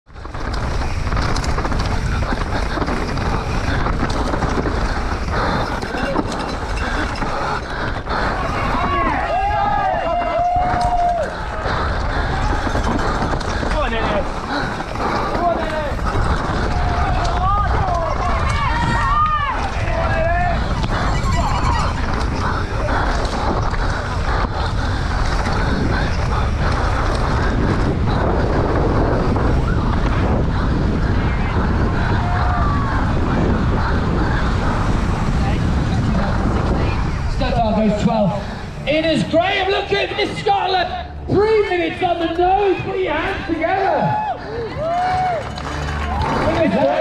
Innes Graham Smashes The EWS Pro Stage
Unbelievable speed as hometown hero Innes Graham smashes his way down Innerleithen to take victory in the men's pro stage on day 1 of the Tweed Valley Enduro World Series

For more EWS Coverage head to the EWS Youtube Channel or catch the Highlights on our youtube this evening!